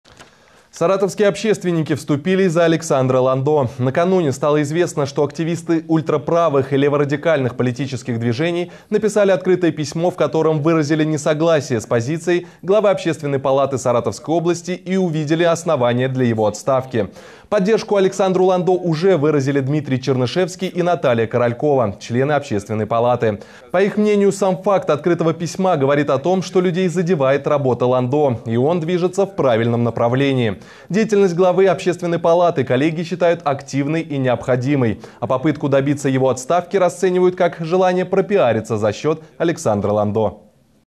Саратовские общественники вступились за Александра Ландо
Накануне стало известно, что активисты ультраправых и леворадикальных политических движений написали открытое письмо, в котором выразили несогласие с позицией главы общественной палаты Саратовской области и увидели основания для его отставки. Подробнее смотрите в материале от 2 декабря 2015 года.